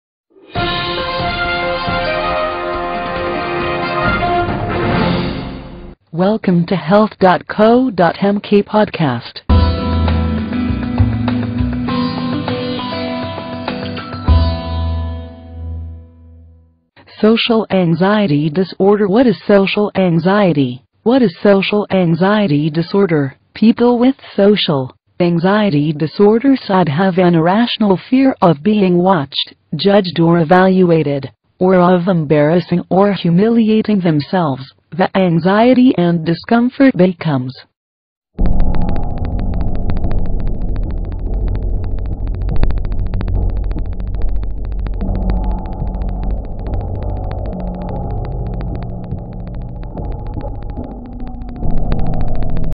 Social Anxiety Disorder - What is Social Anxiety Disorder
A good starting point for learning about social anxiety disorder, this article briefly covers all of the key elements of the disorder including symptoms, diagnosis, and treatment.